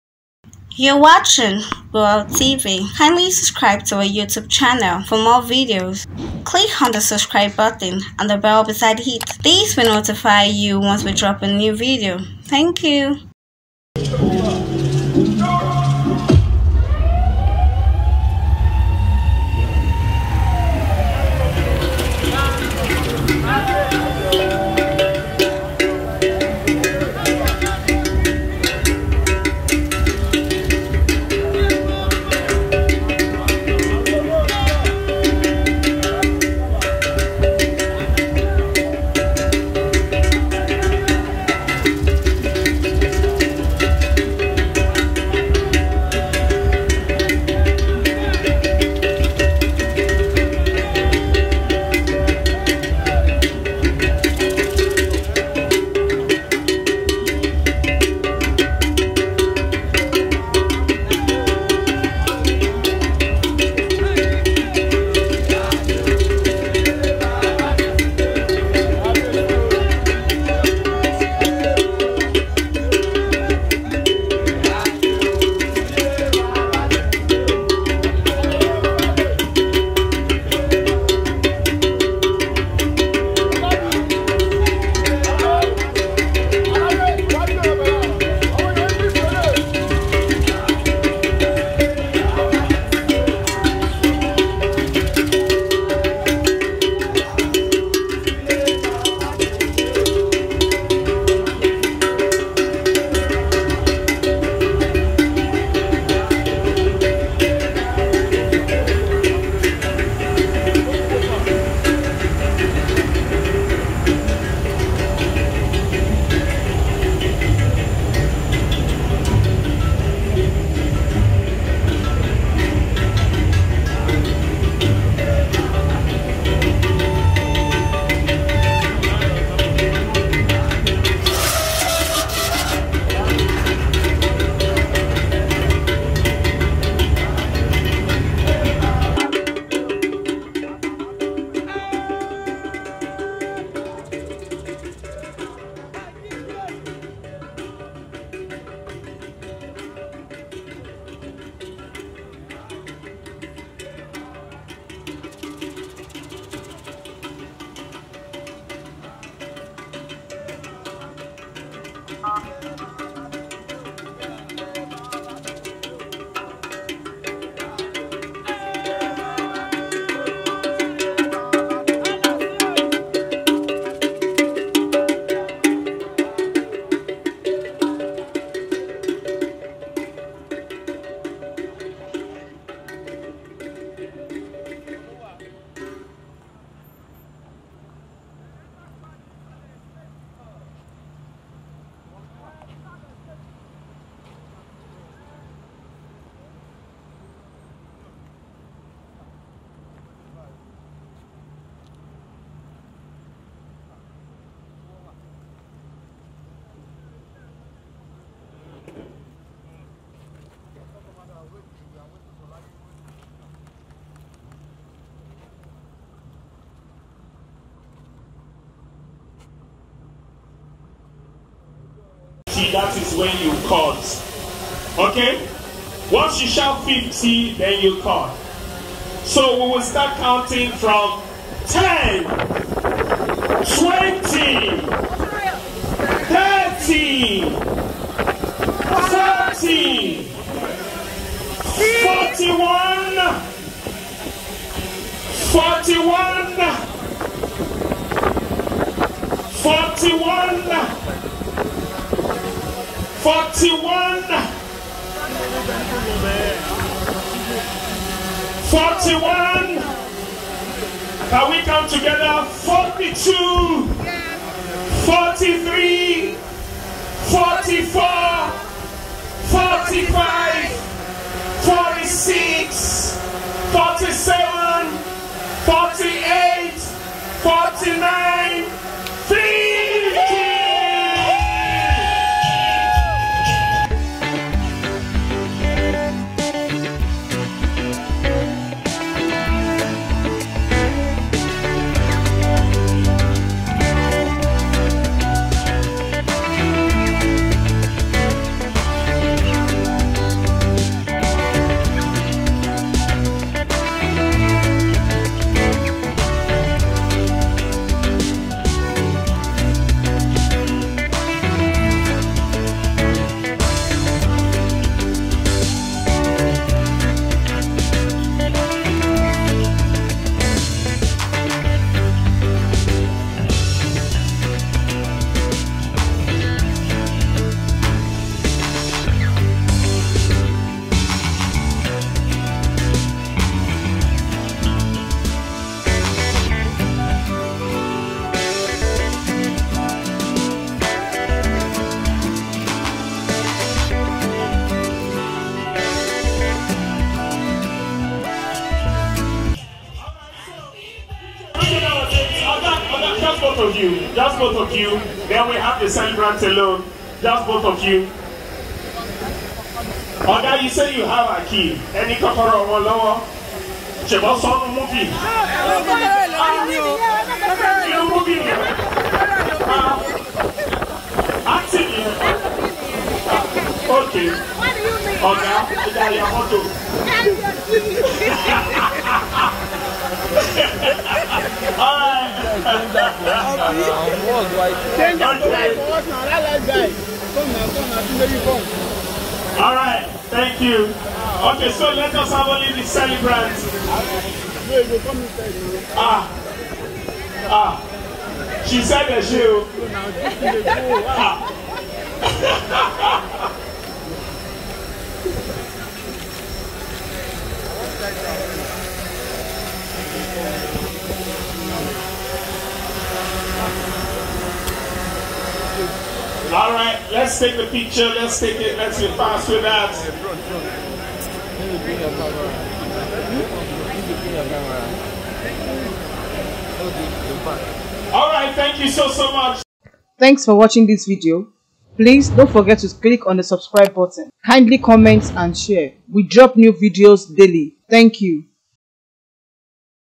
Newly Acquired Bentley Ooni Of Ife Brought To His Billionaire Friend Party As The Celebrant Cut Cake
See The Newly Acquired Bentley Ooni Of Ife Brought To His Billionaire Friend Party As The Celebrant Cut Cake.